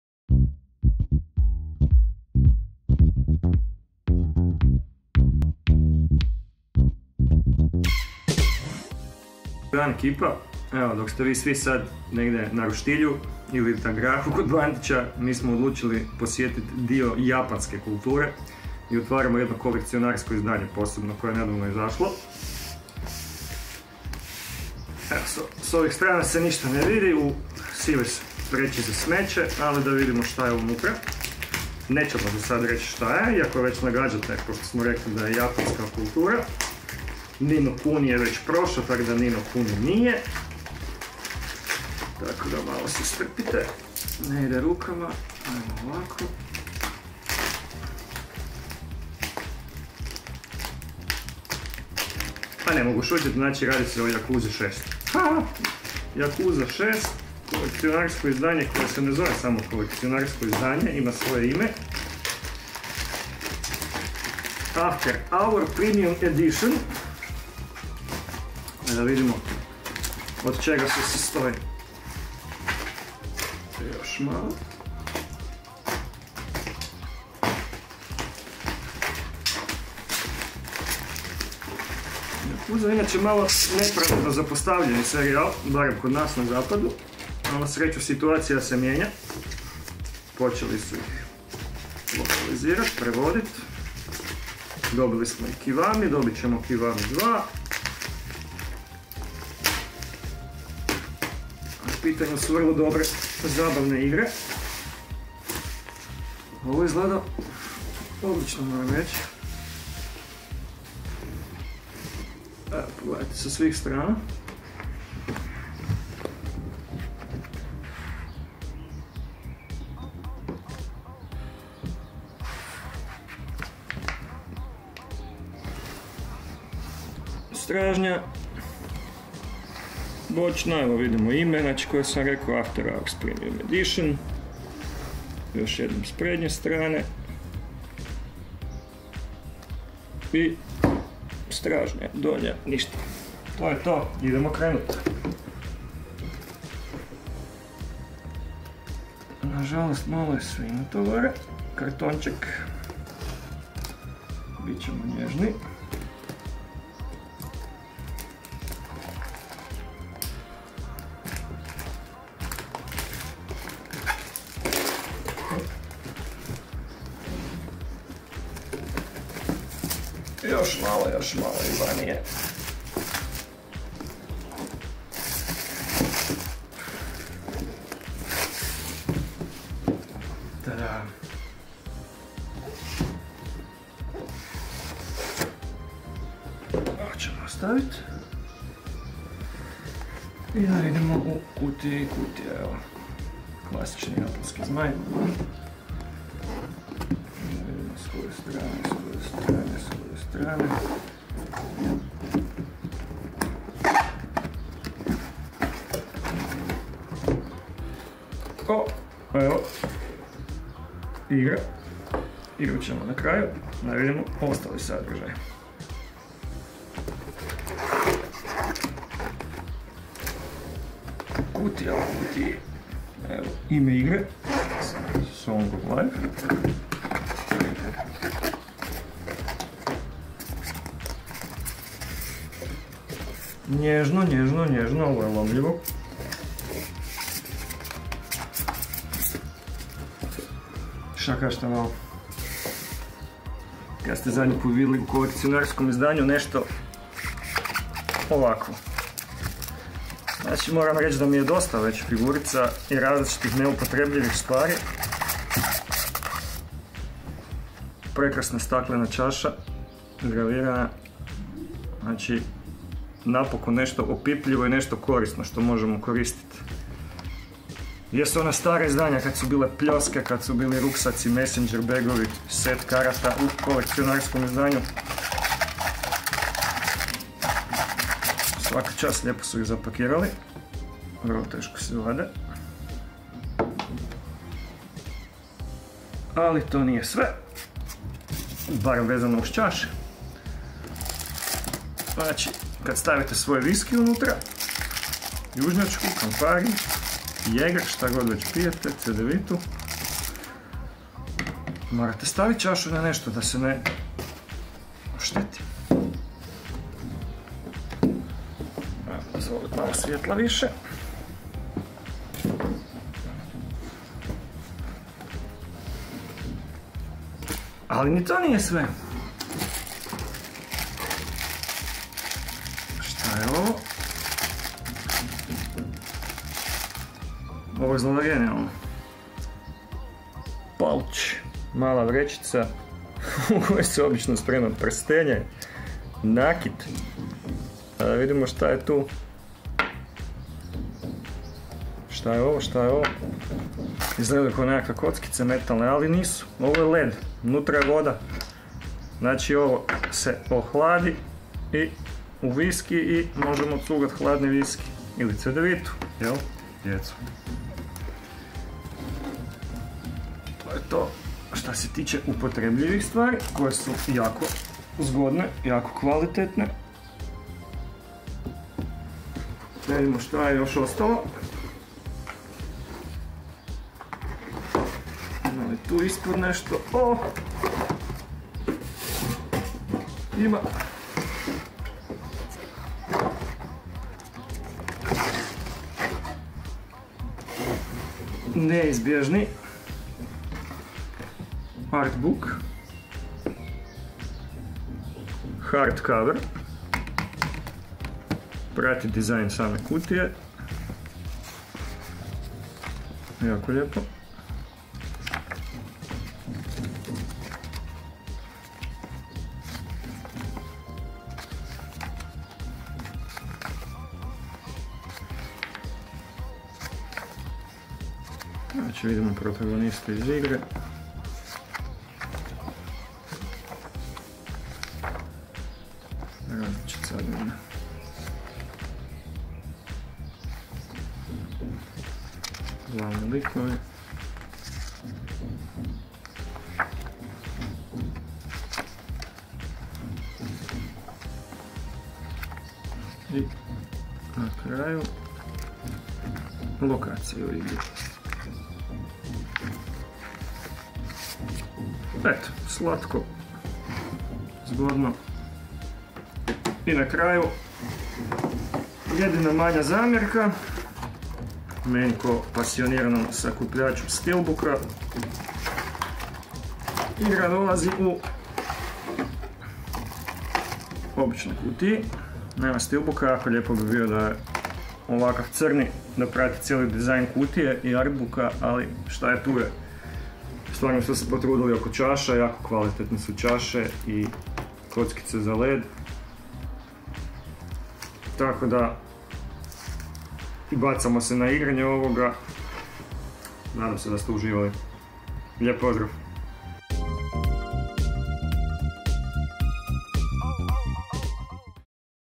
Otpakiravamo Yakuza 6: The Song of Life - After Hours Premium Edition || FFA.hr
Otpakirali smo kolekcionarsko izdanje Yakuze 6 i moramo priznati da nam se sviđa što sadrži konkretne stvari poput čaša i podmetača. Već se vidimo kako se po nadolazećim vrućinama hladimo uz omiljeno piće!